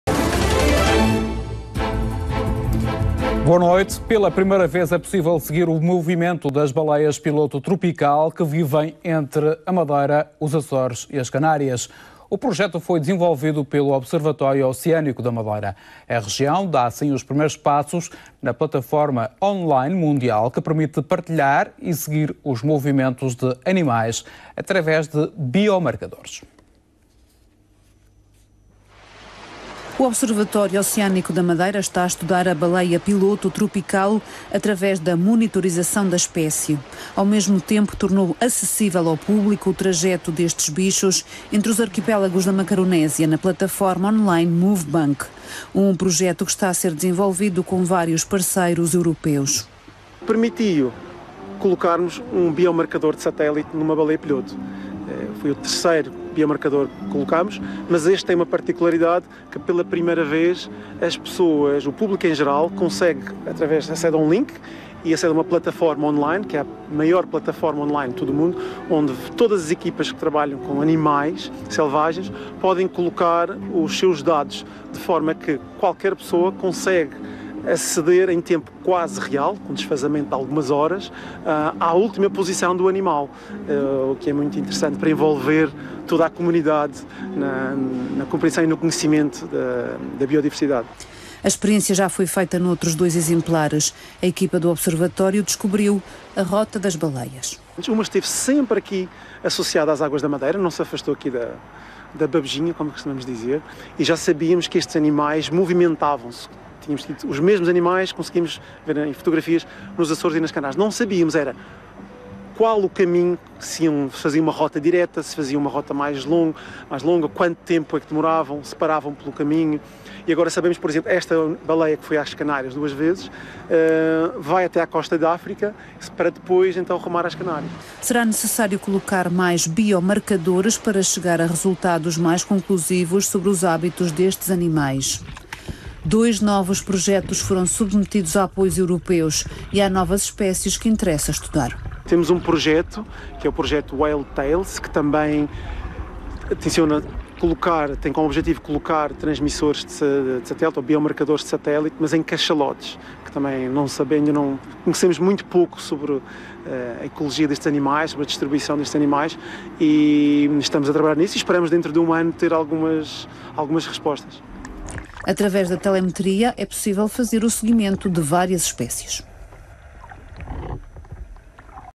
RTP - Madeira | Marcação de uma baleia-piloto-tropical na Madeira
Uma equipa de biólogos marinhos do Observatório Oceânico da Madeira conseguiu colocar a Madeira no mapa do MoveBank, uma plataforma internacional para a partilha da monitorização de animais selvagens. Através da marcação de uma baleia-piloto-tropical (Globicephala macrorhynchus), a partir de agora o público em geral pode seguir o tracking deste animal, marcado com um transmissor de satélite durante os trabalhos de campo do mês de maio.